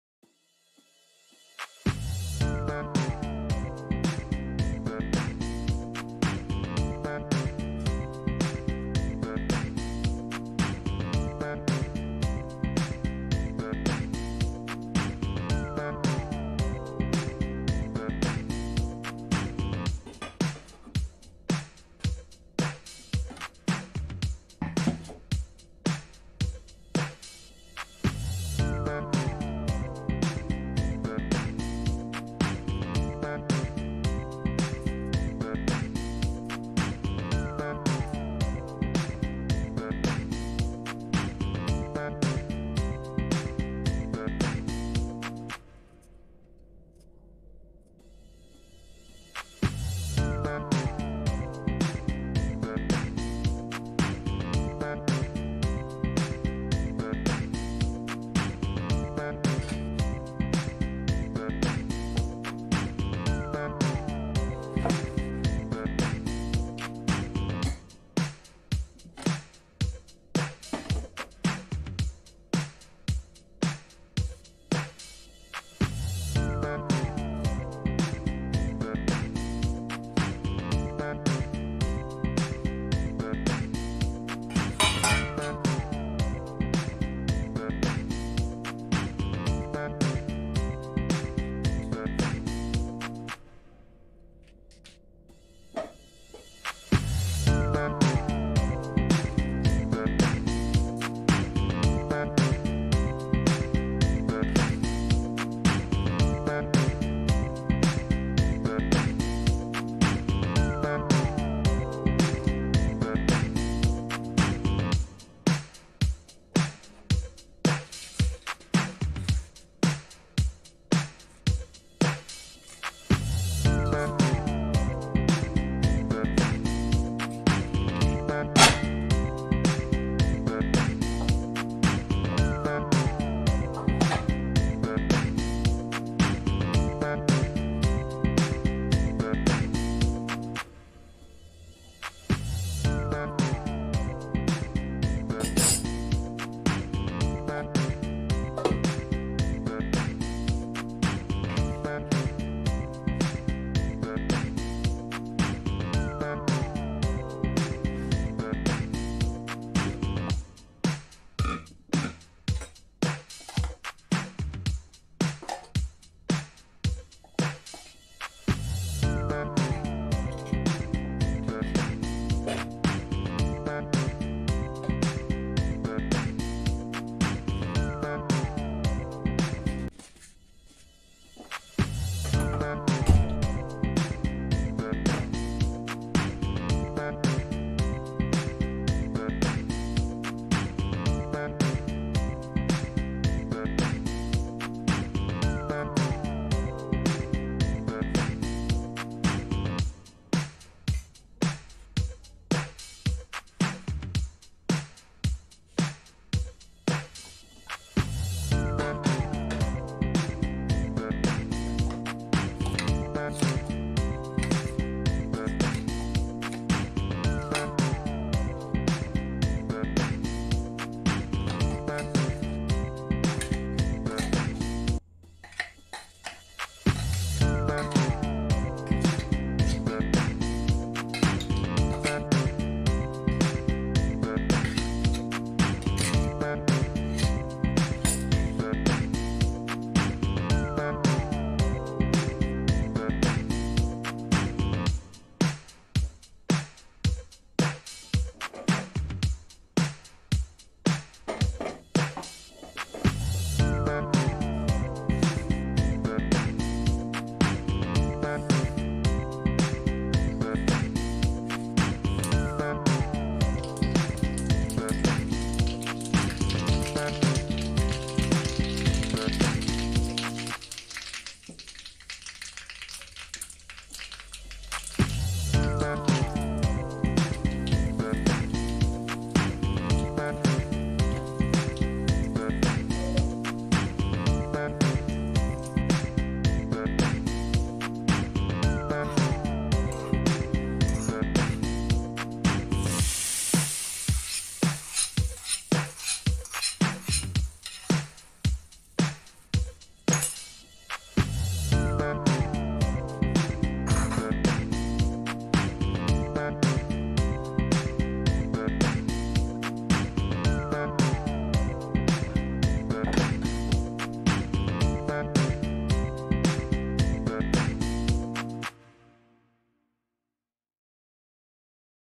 To increase memory power Brahmi leaves Chutney
ingredients - chopped Brahmi leaves one cup, grated coconut 🥥 one cup, pepper one tsp, cumin seeds one tsp, chilli powder half tsp, Salt one tsp, tamarind gooseberry size, jaggery two tsps For tempering - oil one tsp, mustard seeds one tsp, cumin seeds one tsp, asafoetida half tsp, curry leaves For Tambuli- half cup curd or one cup butter milk